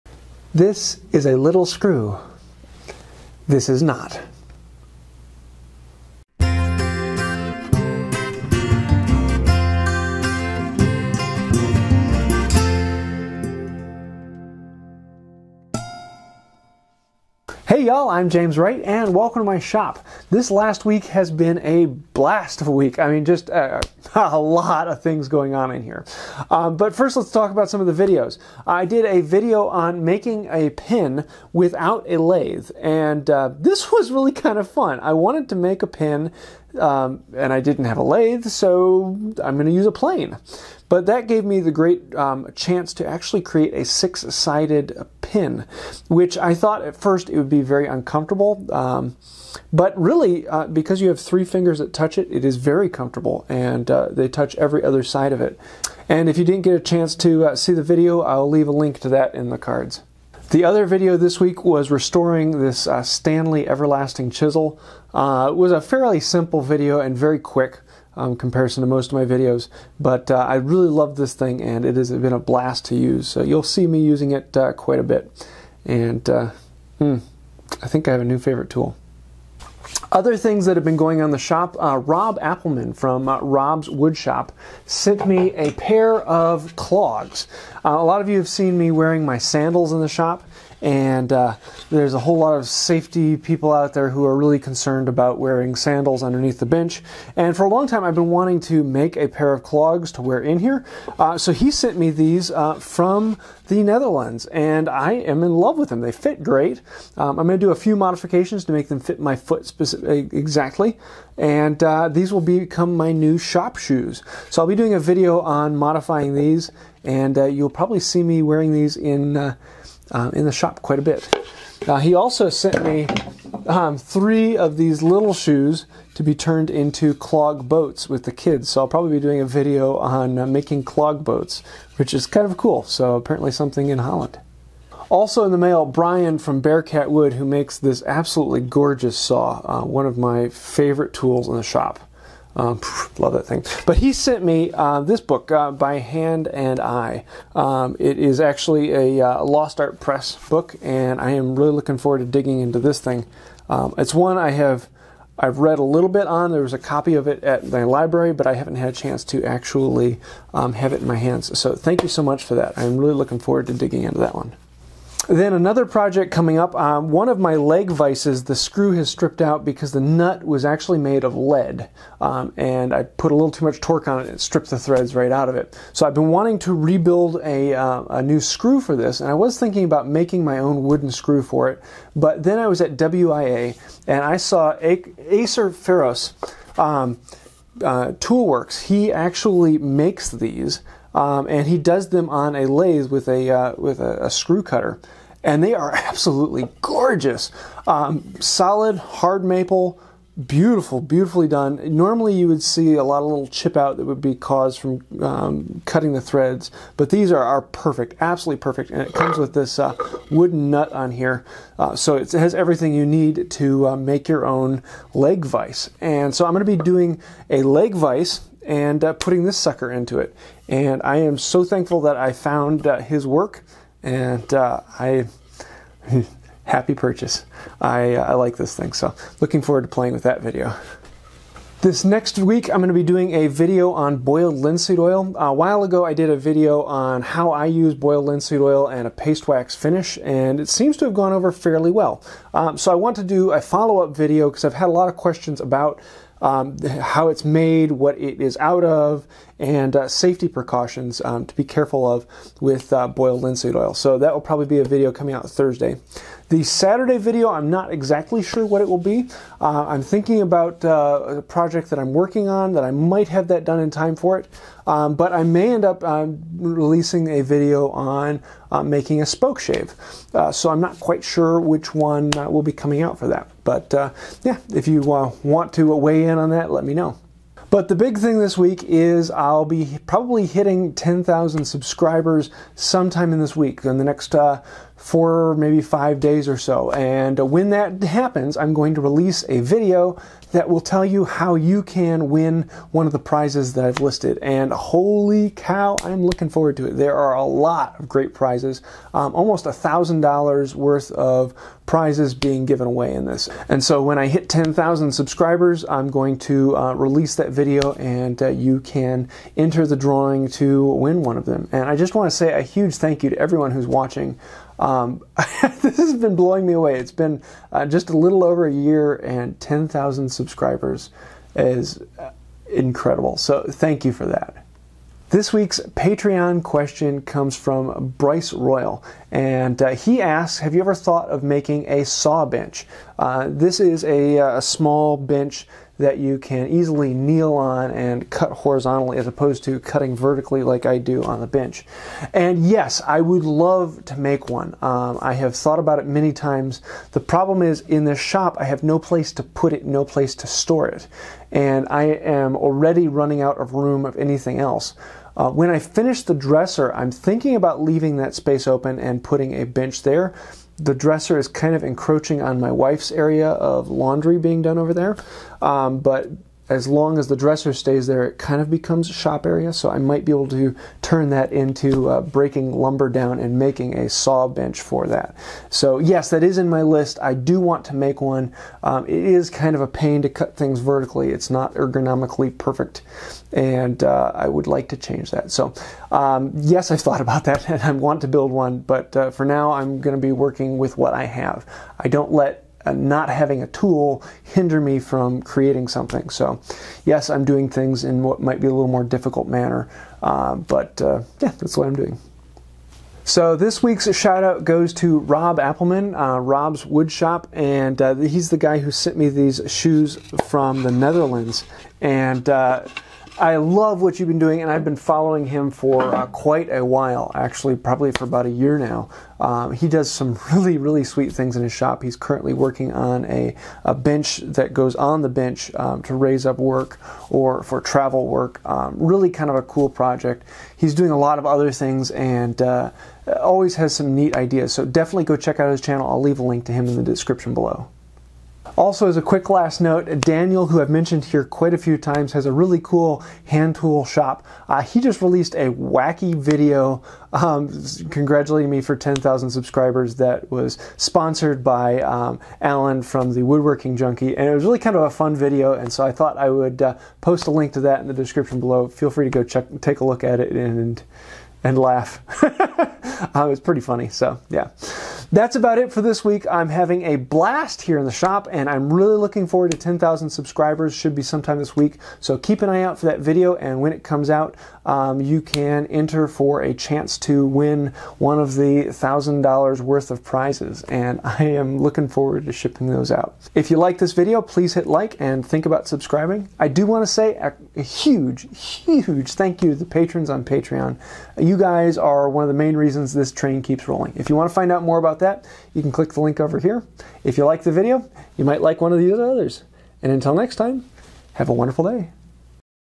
Report, Chisel Restore, Pen Without a Lathe, 10K Giveaway, Clogs, and more
This last week was fun but I am looking forward to doing the 10K giveaway this week!

You Can find me:
Beme: WoodByWright
Periscope: @WoodByWright